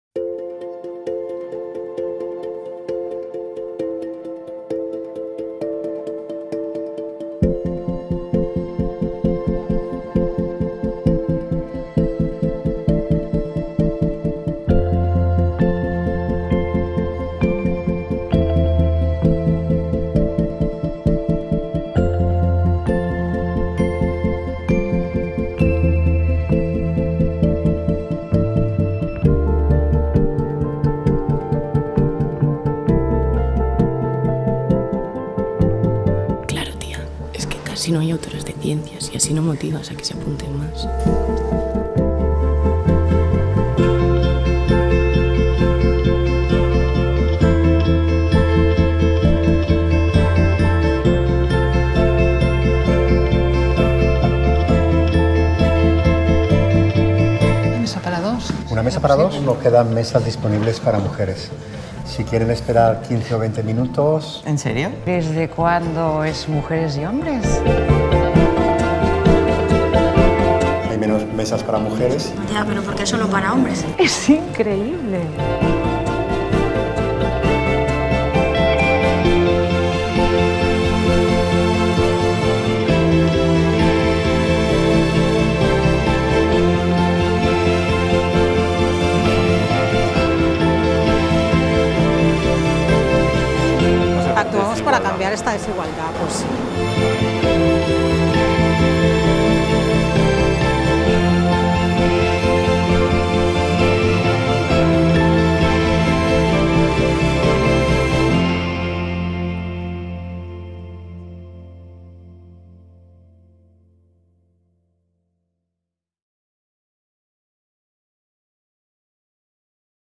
‘Igualdad de género_Wengage’, de Ogilvy Barcelona para Caixabank
Ficha técnica de la campaña ‘Igualdad de género_Wengage’

Anunciante: CaixaBank
Marca: CaixaBank
Producto: Banca
Responsables del  anunciante: Sílvia Jové y Oscar López de Murillas
Agencia: Ogilvy Barcelona
Subdirectora general: Gemma Gutiérrez Cortés
Directora de cuentas: Núria Martí Mayral
Ejecutiva de cuentas: Maria Carbó López
Director creativo ejecutivo: Gabriel García de Oro
Director creativo y redactor: Jofre Banquells i Fernández
Directora de arte: Natalia Solana Racero
Producer: Ingrid Calvelo
Productora: Producciones Oxígeno
Realizador: Ivan Garriga Simó
Sonido y post-producción: Pijama Studio
Fecha de inicio de campaña: 11 de diciembre de 2018